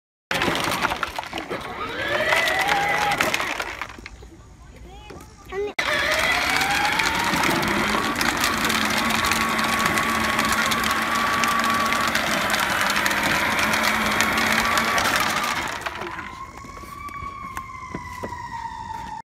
A Fun and a Short Ride on an ATV
The girls are trying out the Atv trying to see who goes the longest and who likes this ride the most!